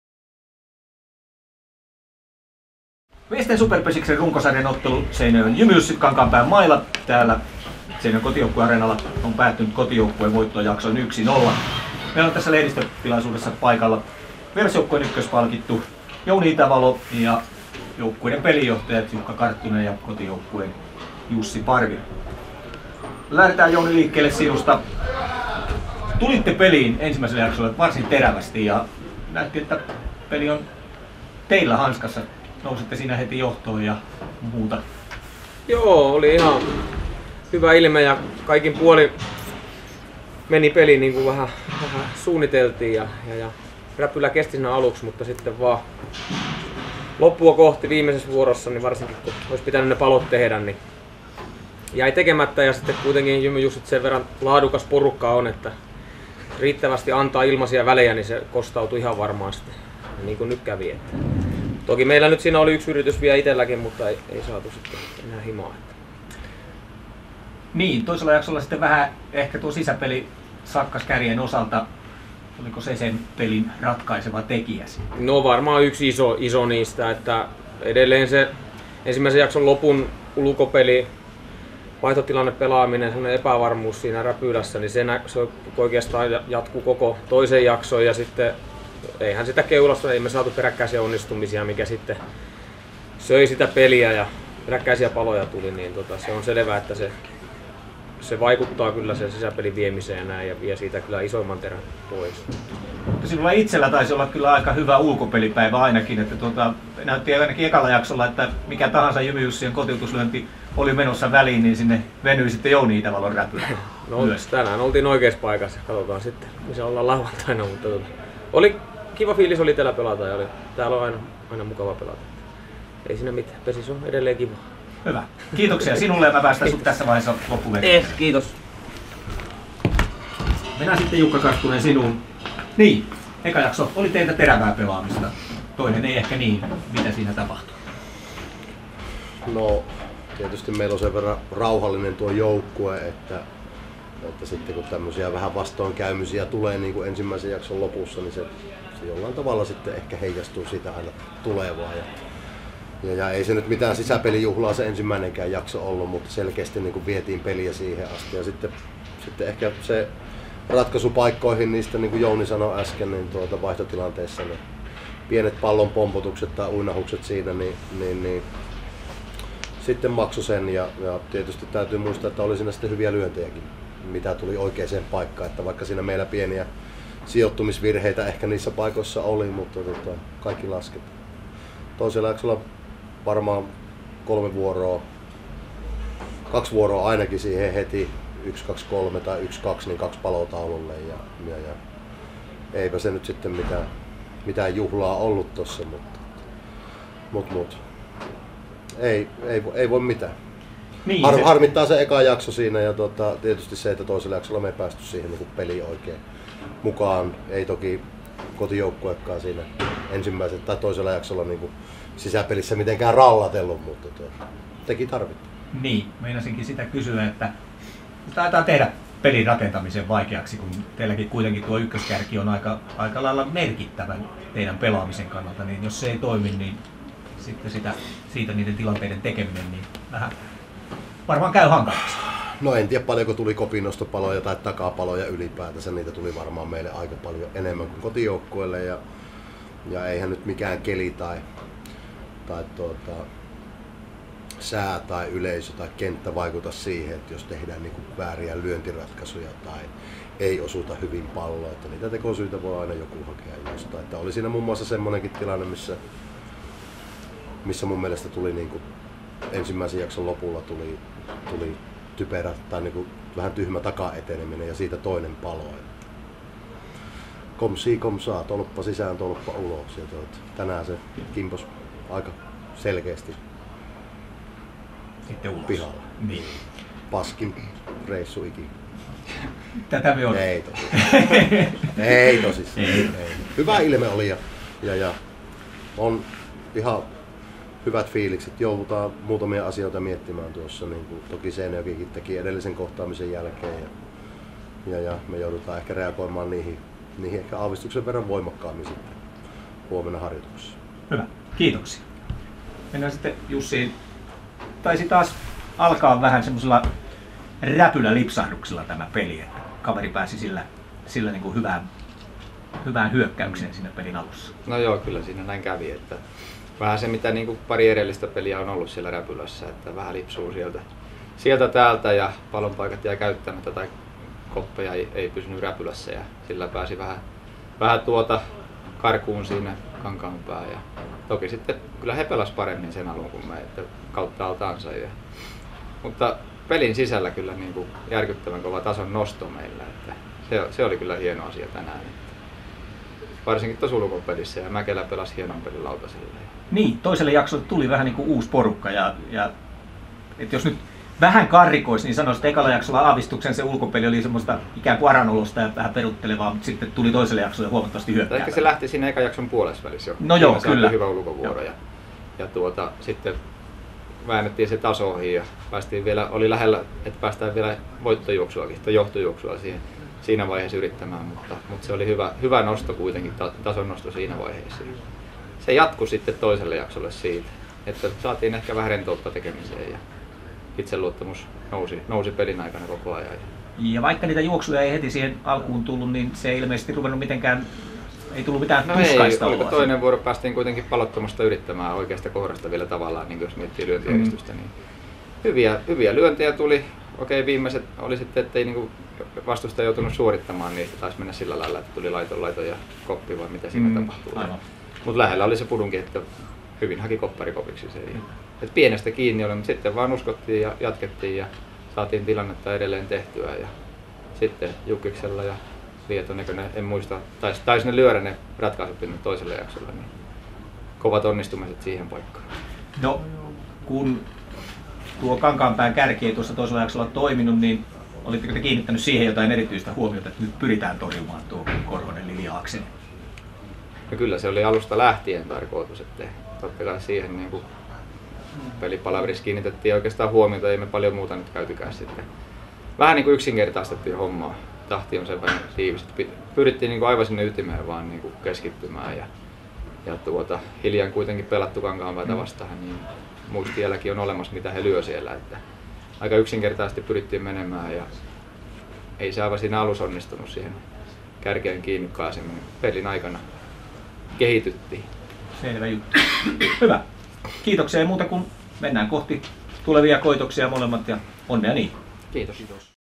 Lehdistötilaisuus: JymyJussit - Kankaanpään Maila 23.7.
Lehdistötilaisuuden tarjoaa Atria! Paikalla pelinjohtajat, Kankaanpään Mailan Jukka Karttunen ja JymyJussien Jussi Parvi. Lisäksi ykköspalkituista paikalla KaMan Jouni Itävalo. JymyJussien ykköspalkittu J-P Vainionpää ei valitettavasti hoksannut tulla paikalle, vaikka jokunen toisto miehellä yläparvelle saapumisesta on urallaan. 😀